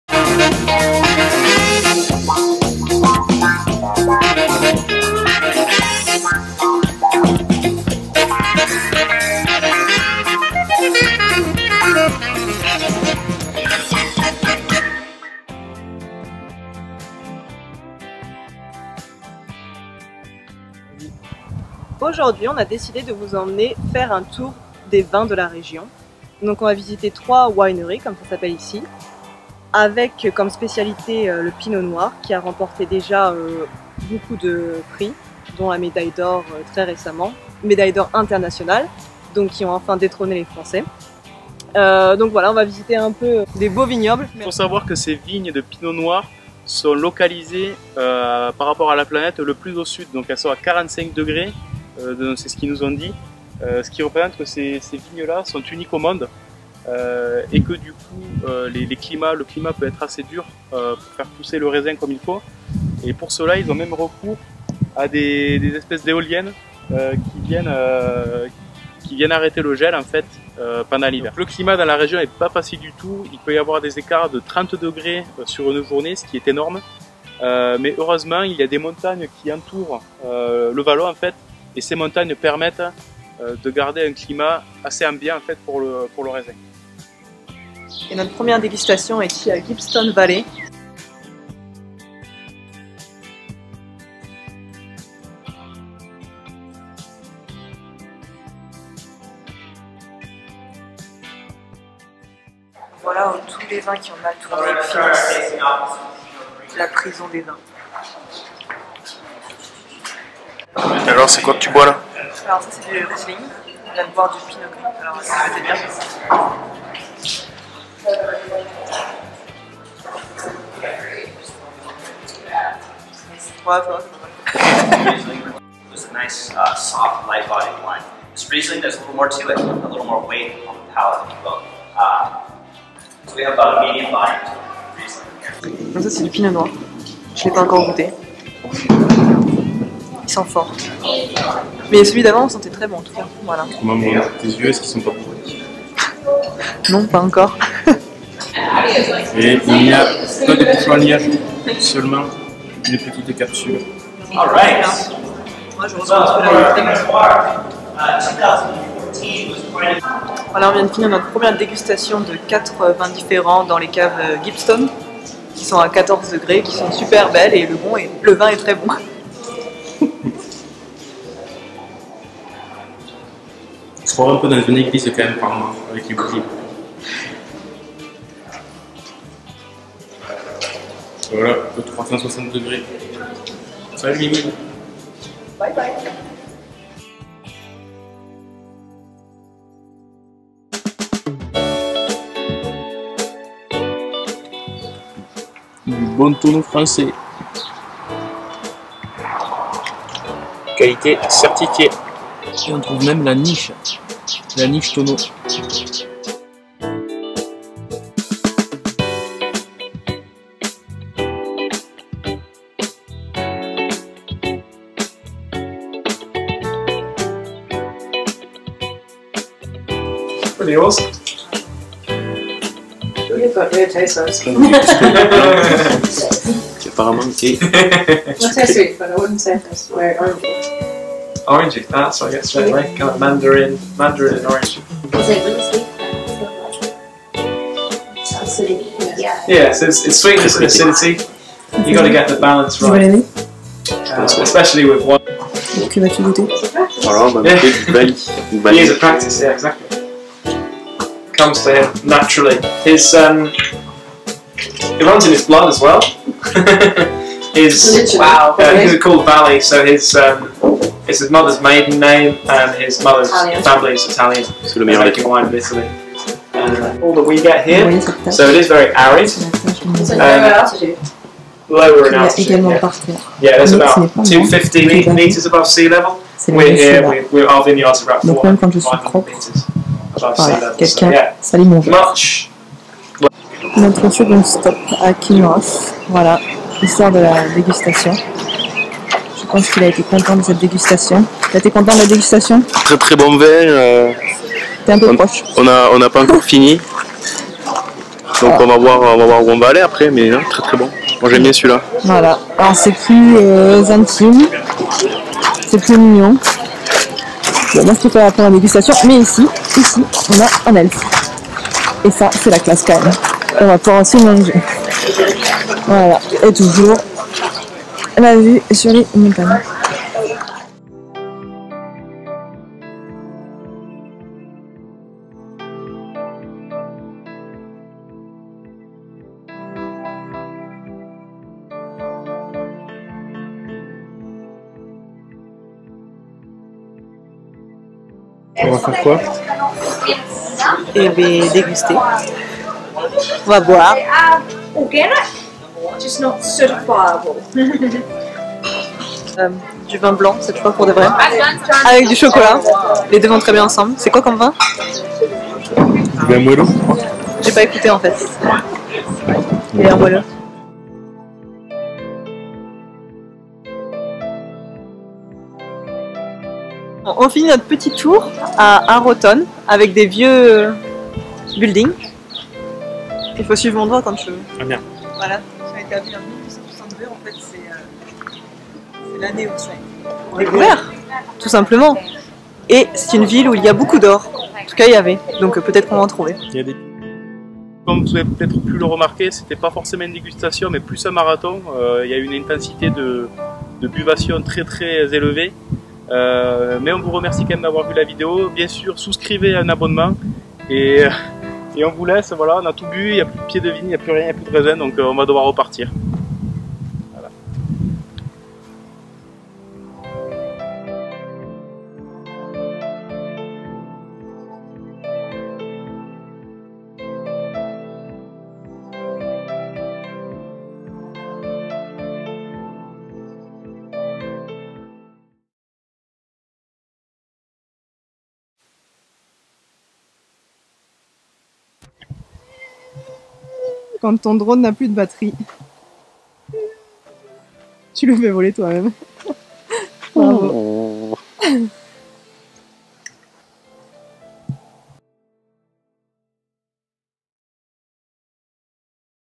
VLOG NOUVELLE-ZELANDE #12 : WINE TOUR QUEENSTOWN OTAGO
On a gouté les vins Néo Zélandais de la région de Gibbston en commençant par la cave de Gibbston Valley, puis le domaine de Kinross, et pour finir, le domaine de Akarua.

Bon visionnage :)

Mimine & Minou